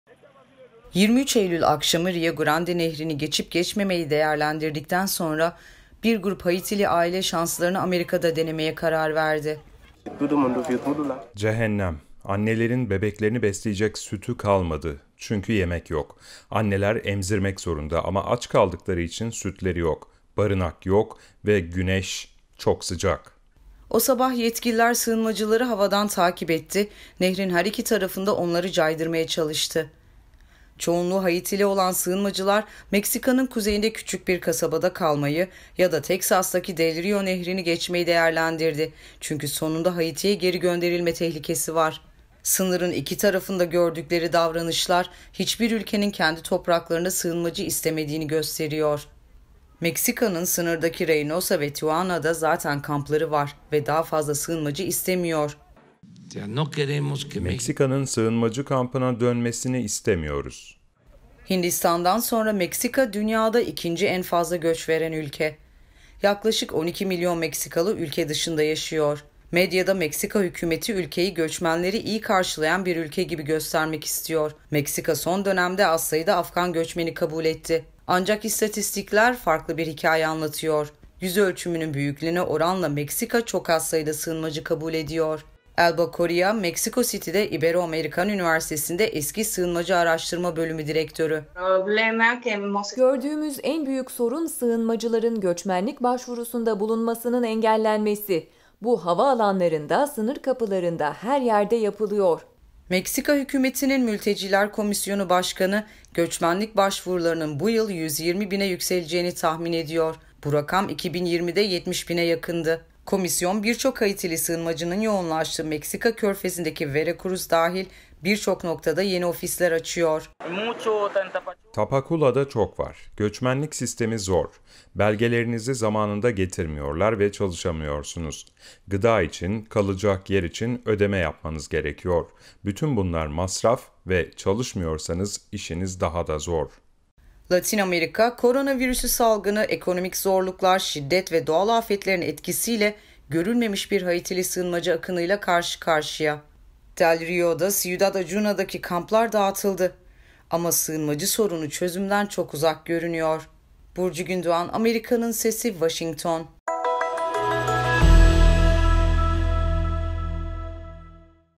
Binlerce Haitili Göçmen Meksika Sınırında
Binlerce sığınmacı Güney Amerika’dan gelip Meksika-Amerika sınırına yığılmış durumda